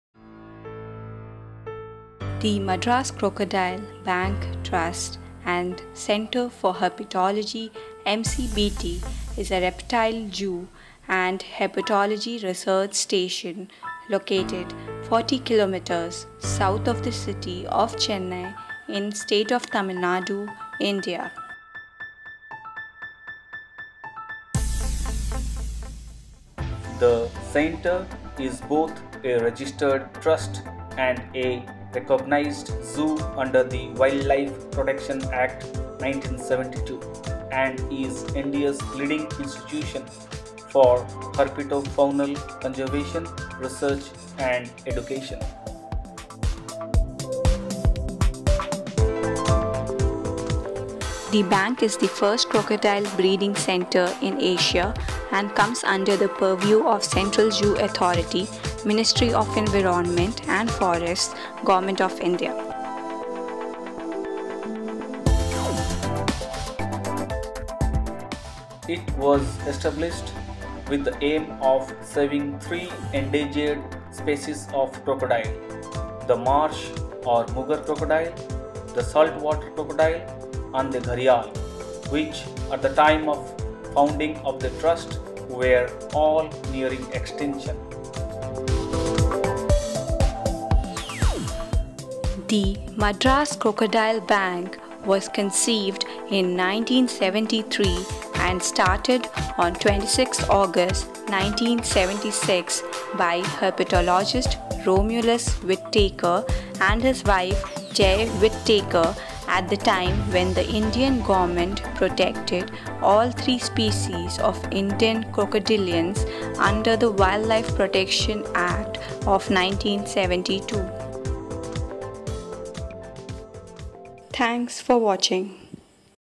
Crocodile Bank
The Madras Crocodile Bank Trust and Centre for Herpetology (MCBT) is a reptile zoo and herpetologyresearch station, located 40 kilometres (25 mi) south of the city of Chennai, in state of Tamil Nadu, India.

The centre is both a registered trust and a recognized zoo under the Wildlife (Protection) Act, 1972 and is India's leading institution for herpeto faunal conservation, research and education.

The bank is the first crocodile breeding centre in Asia and comes under the purview of the Central Zoo Authority, Ministry of Environment and Forests, Government of India.

It was established with the aim of saving three Indian endangered species of crocodile—the marsh or mugger crocodile, the saltwater crocodile, and the gharial, which at the time of founding of the trust were all nearing extinction.

The Madras Crocodile Bank was conceived in 1973 and started on 26 August 1976 by herpetologist Romulus Whitaker and his wife Zai Whitaker at the time when the Indian government protected all three species of Indian crocodilians under the Wild Life Protection Act of 1972.